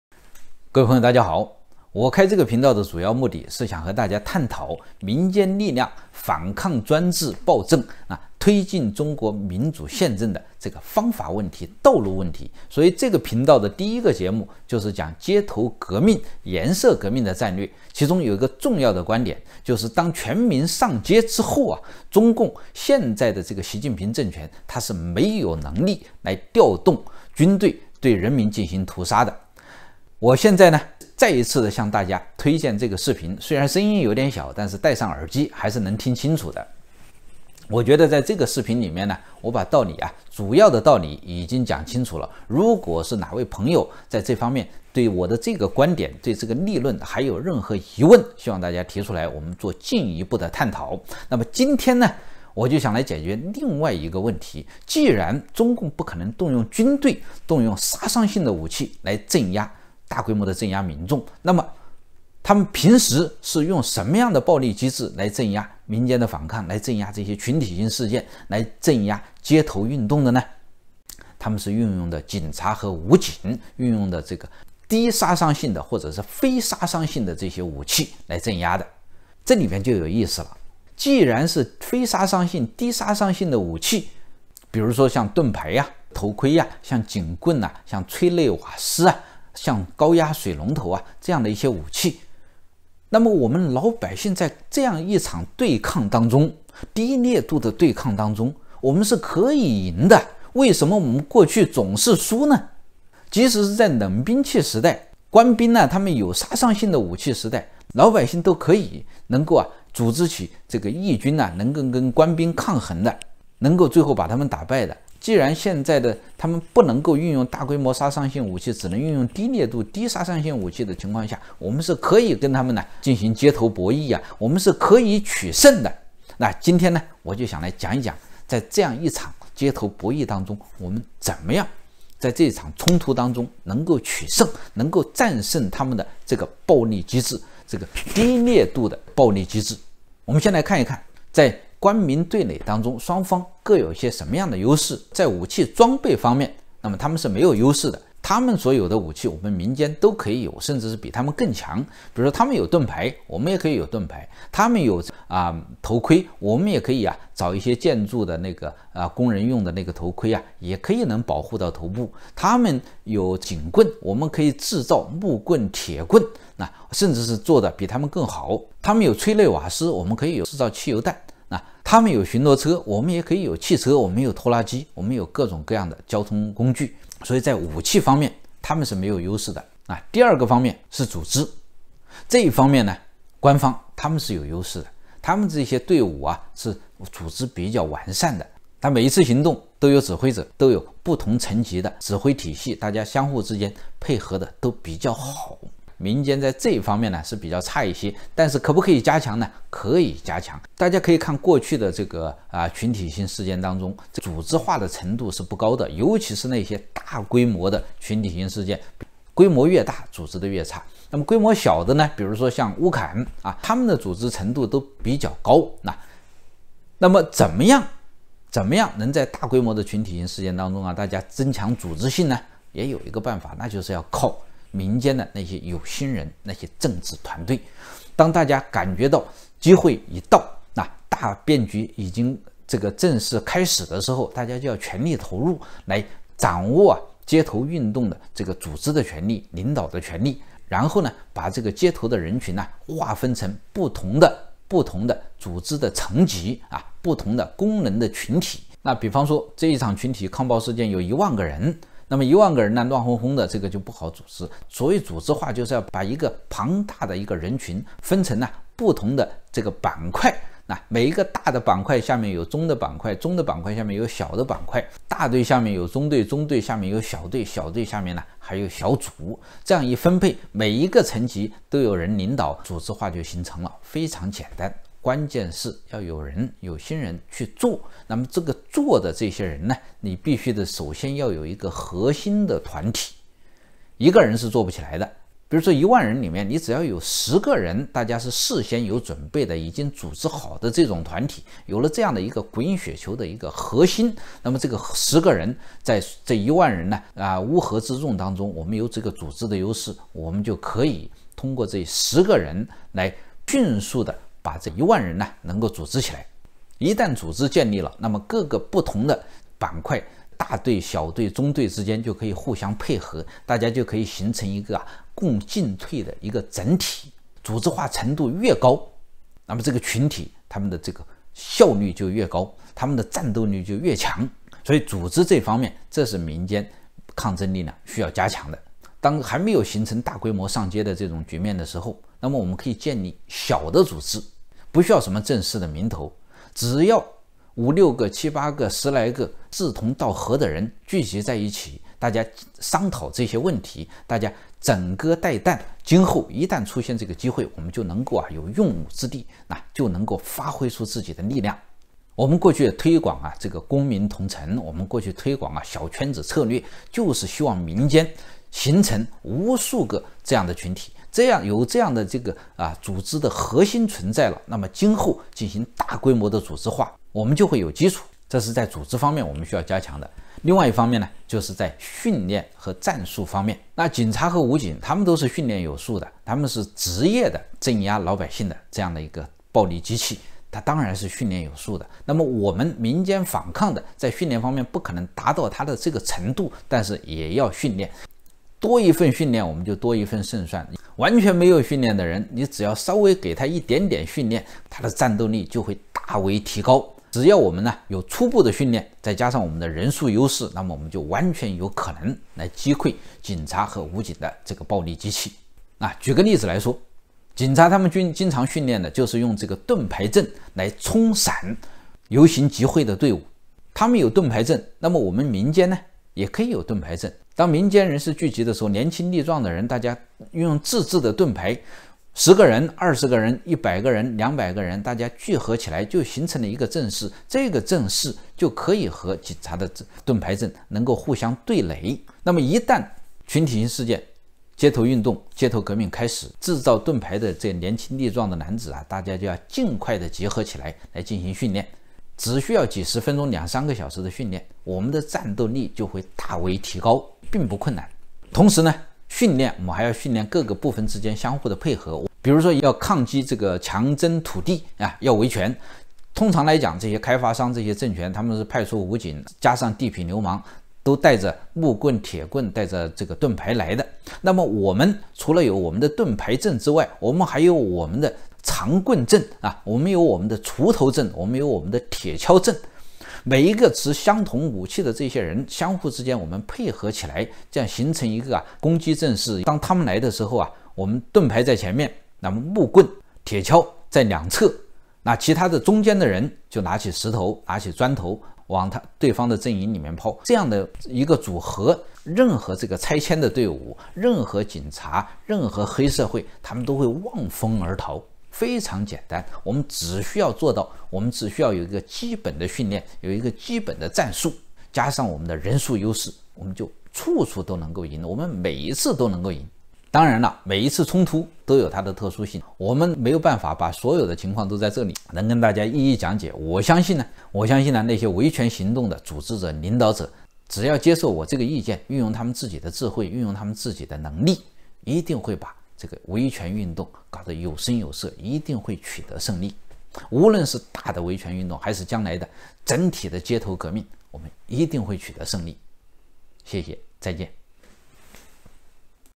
只要领导者接受这几个建议，维权群体就可以完败警察武警！（一平论政97）
从装备、组织、训练以及人数等几个方面来看，维权民众完全可以完败警察武警！
1，中共维稳不敢大规模动用军队和杀伤性武器，只用警察武警黑社会，以非杀伤性武器为主。维权民众可以轻易在武器上取得优势。
2，维稳队伍组织化和训练程度高，但是维权民众也可以迅速提高组织化和训练程度。需要有心人去准备。
3，维权民众有人数优势。尤其是今后全民上街之后。
这几方面实力对比，说明只要民间抗争阵营有一批做实事的有心人，维权群体就能赢！街头博弈就能赢。

全文：我开这个频道的主要目的是想和大家探讨民间力量反抗专制暴政，推进中国民主宪政的这个方法问题，道路问题，所以这个频道的第一个节目就是讲街头革命，颜色革命的战略。其中有一个重要的观点，就是当全民上街之后，中共现在这个习近平政权，他是没有能力来调动军队对人民进行屠杀的。我现在呢，再一次向大家推荐这个视频，虽然声音有点小，但是戴上耳机还是能听清楚的。我觉得在这个视频里面我把道理，主要的道理已经讲清楚了，如果是哪位朋友在这方面对我的这个观点，对这个立论还有任何疑问，希望大家提出来，我们做进一步的探讨。那么今天呢，我就想来解决另一个问题，既然中共不可能动用军队，动用杀伤性的武器来大规模的镇压民众，那么，他们平时用什么样的暴力机制来镇压民间的反抗？来镇压这些群体性事件？来镇压街头运动的呢？

他们是运用的警察和武警；运用的这个低杀伤性的，或者是非杀伤性这些武器来镇压的。这里面就有意思了，既然是非杀伤性，低杀伤性的武器，比如说象盾牌呀；头盔；象警棍；象催泪瓦斯；象高压水龙头这样的一些武器，那么我们老百姓在这样一场对抗当中，低烈度的对抗当中，我们是可以赢的。为什么我们过去总是输呢？即使是在冷兵器时代，官兵他们有杀伤性武器的时代，老百姓都可以能够组织起义军能够跟官兵抗衡的，能够最后把他们打败的。既然现在他们不能够运用大规模杀伤性武器，只能运用低烈度的 '低杀伤性武器的情况下，我们是可以跟他们进行街头博弈，我们是可以取胜的。

今天呢，我就想来讲一讲这样一场街头博弈当中，我们怎么样在这场冲突当中能够取胜？能够战胜他们的暴力机制？这个低烈度的暴力机制？我们现在来看一看在官民对垒当中，双方各有什么样的优势？在武器装备方面，那么他们是没有优势的，他们所有的武器，我们民间都可以有，甚至是比他们更强。比如他们有盾牌，我们也可以有盾牌；他们有头盔，我们也可以呀，找些建筑的那个工人用的头盔，也可以保护到头部；他们有警棍，我们可以制造木棍'铁棍，甚至是做的比他们更好；他们有催泪瓦斯，我们可以制造汽油弹；他们有巡逻车，我们也可以有汽车，我们有推拉机，我们有各种各样的交通工具, 所以在武器方面，他們是沒有優勢的。

第二个方面，是组织，这一方面官方他们是有优势的，他们这些队伍是组织比较完善的，他每一次行动都有指挥者，都有不同层级的指挥体系，大家相互之间配合得都比较好。民间在这一方面是比较差一些，但是可不可以加强呢？可以加强，大家可以看过去的这个群体性事件当中组织化程度是不高的，尤其是那些大规模的群体性事件，规模越大组织得越差，那么规模小的呢，象乌坎他们的组织程度都比较高。那么怎么样在大规模群体性事件当中，大家增强组织性呢？也有一个办法，那就是要靠民间的有心人，那些政治团队。

当大家感觉到机会已到，大变局已经正式开始的时候，大家就要全力投入来掌握街头运动的这个组织的权力；领导的权力，然后呢，把这个街头的人群划分成不同的组织的层级，不同的工人的群体，比方说这一场群体抗暴事件有一万个人，那么一万个人乱哄哄的就不好组织，所以组织化就是要把一个庞大的一个人群分成不同的板块，每一个大的板块下面有中的板块；中的板块下面有小的板块；大队下面有中队；中队下面有小队；小队下面还有小组，这样一分配每一个层级都有人领导，组织化就形成了，非常简单。关键是要有人有心人去做，那么做的这些人，你必须要有核心的团体，一个人是做不起来的，比如说一万人里面你只要有十个人大家是事先有准备的，已经组织好的这种团体，有了这样一个滚雪球的核心，那么这个十个人在一万人乌合之众当中，我们有这个组织的优势，我们就可以通过这十个人来迅速的把这一万人能够组织起来。

一旦组织建立了，那么各个不同的板块，大队 小队 中队之间就可以互相配合，大家就可以形成一个共进退的一个整体。组织化程度越高，那么这个群体他们这个效率就越高，他们的战斗力就越强，所以组织这方面，这是民间抗争力量需要加强的。当还没有形成大规模上街的这种局面的时候，那么我们可以建立小的组织，不需要什么正式的名头，只要五六个，七八个，十来个志同道合的人聚集在一起，大家商讨这些问题，大家枕戈待旦，今后一旦出现这个机会，我们就能够有用武之地，就能够发挥出自己的力量。我们过去推广公民同城；我们过去推广小圈子策略，就是希望民间形成无数个这样的群体。有这样的组织核心存在了，那么今后进行大规模的组织化，我们就会有基础，那是在组织方面需要加强的。

另外一方面呢，就是在训练和战术方面，那警察和武警他们都是训练有素的，他们是职业的镇压老百姓的这样一个暴力机器，他当然是训练有素的。那么我们民间反抗的在训练方面不可能达到他这个程度，但是也要训练，多一份训练，我们就多一份胜算。完全没有训练的人，只要稍微给他一点点训练，他战斗力就会提高。只要我们有初步的训练，再加上我们的人数优势，那么我们就完全有可能来击溃警察和武警的这个暴力机器。举个例子来说，警察他们经常训练的就是用这个盾牌阵来冲散游行集会的队伍，他们有盾牌阵，那么我们民间呢？也可以有盾牌阵，当民间人士聚集的时候，年轻力壮的人大家用自制的盾牌，十个人，二十个人，一百个人，两百个人，大家聚合起来就形成了一个阵势，这个阵势就可以和警察的盾牌阵能够互相对垒。那么一旦群体性事件，街头运动' 街头革命开始，制造盾牌的年轻力壮男子大家就要尽快的结合起来来进行训练。

只需要几十分钟，两三个小时的训练，我们的战斗力就会大为提高，并不困难。同时呢，训练我们还要训练各个部分之间的相互配合，比如说，要抗击这个强征土地要维权，通常来讲这些开发商，这些政权他们是派出武警，加上地痞流氓都带着木棍' 铁棍，带着盾牌来的，那么，我们除了有我们的盾牌阵之外，我们还有我们的长棍阵；我们有我们的锄头阵；我们有我们的铁锹阵。每一个持相同武器的这些人，相互之间我们配合起来，这样形成一个攻击阵势，让他们来的时候，我们盾牌在前面，那么木棍'铁锹在两侧，那其他中间的人就拿起石头，拿起砖头往他们阵营里面抛。这样的一个组合，任何的拆迁队伍；任何警察；任何黑社会，他们都会望风而逃，非常简单。我们只需要做到，我们只需要有一个基本的训练，有一个基本的战术，加上我们的人数优势，我们就处处都能够赢，我们每一次都能够赢。当然了，每一次冲突都有它的特殊性，我们没有办法把所有的情况都在这里能跟大家一一讲解，我相信那些维权行动的组织者，领导者只要接受我这个意见，运用他们自己的智慧，运用他们自己的能力，一定会把维权运动打得有声有色，一定会取得胜利。无论是大的维权运动；还是将来的整体的街头革命，我们一定会取得胜利。